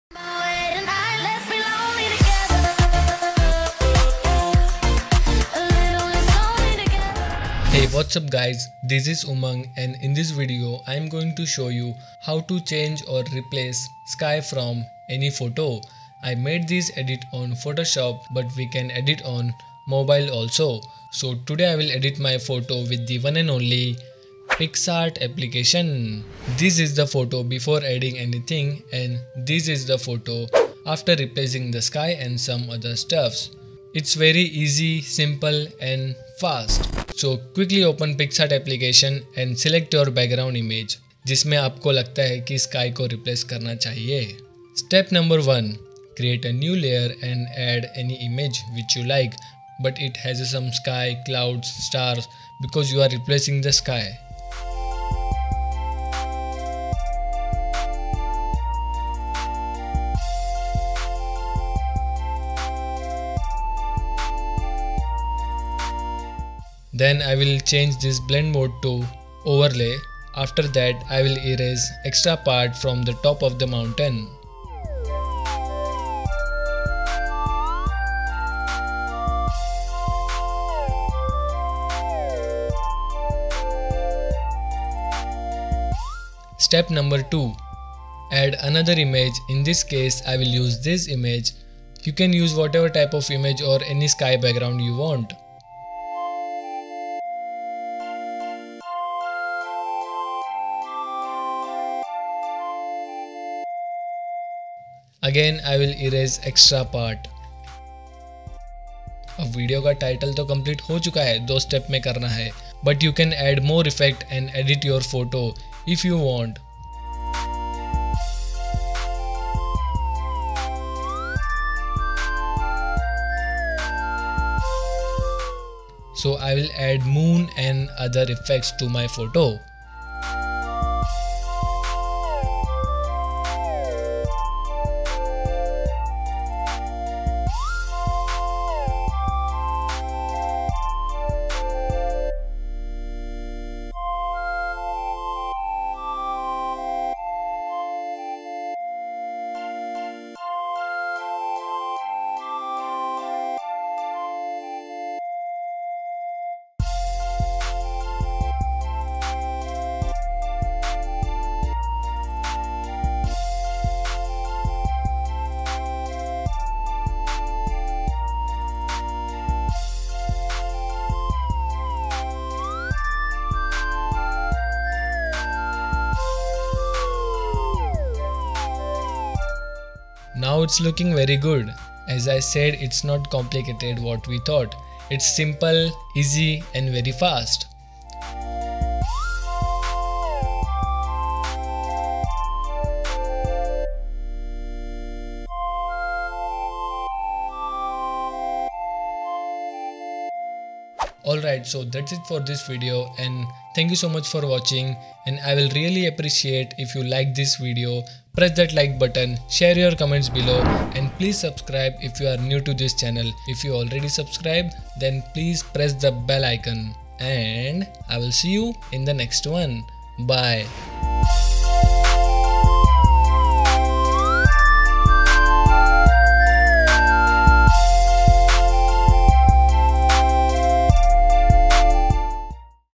Replace the SKY in ANY Photo using PICSART 🔥 [ 2 Steps ]
Hey guys..In this video i'll show you how you can replace sky of any background using picsart.
Hope this video helpful to you. If yes please show some love :-)